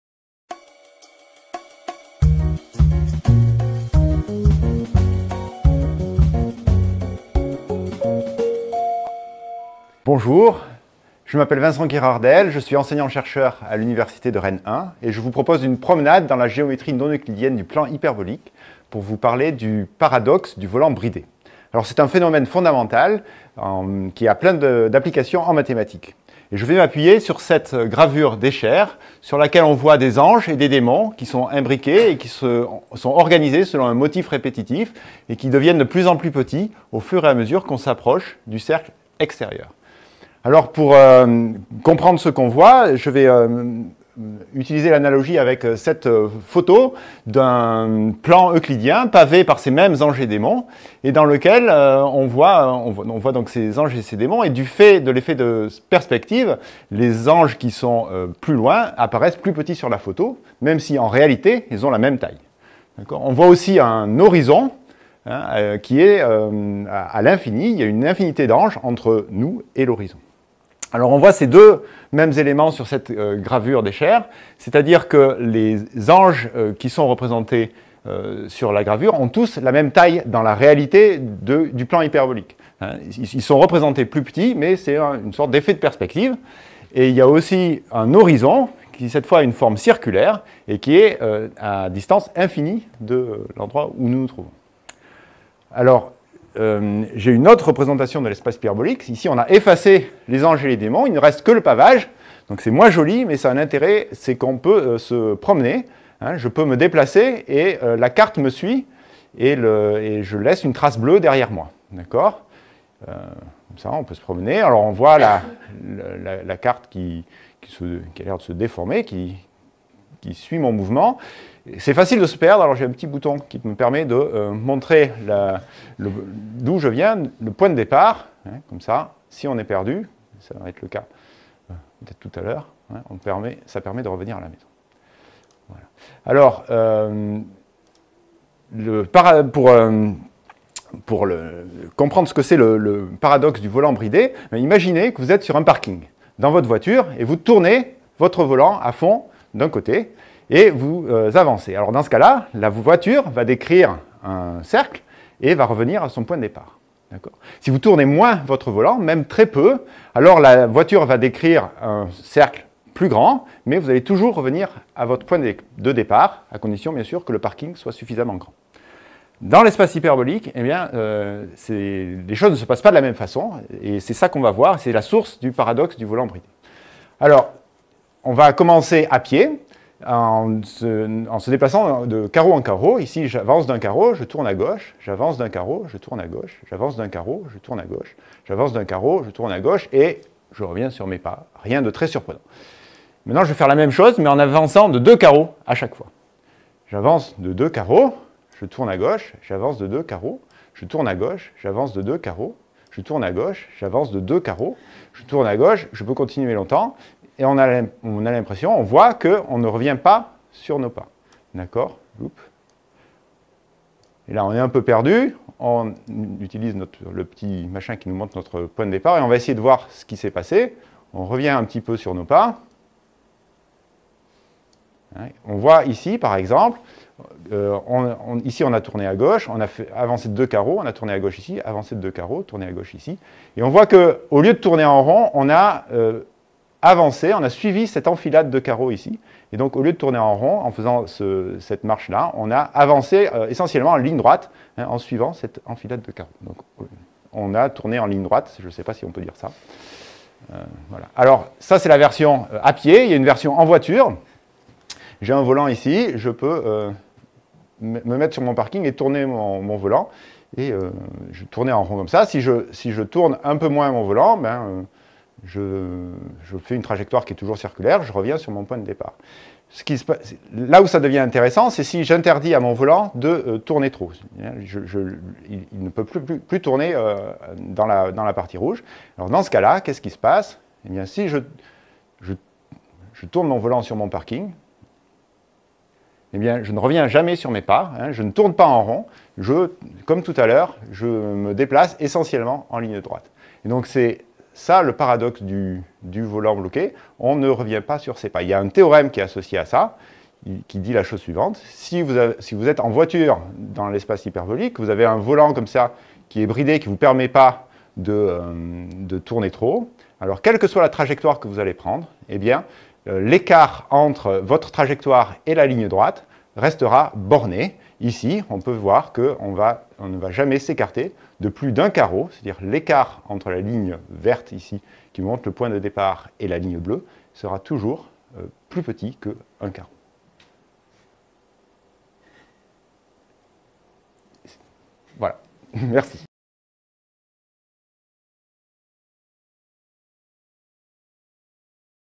Vincent Guirardel - Promenade hyperbolique
le plan hyperbolique est un exemple très important de géométrie non-euclidienne. On va se promener dans cet espace et voir que même si on pense tourner en rond, on va presque tout droit... Ce phénomène dit de "stabilité des quasigéodésiques" est fondamental pour comprendre une large classe d'espaces géométriques.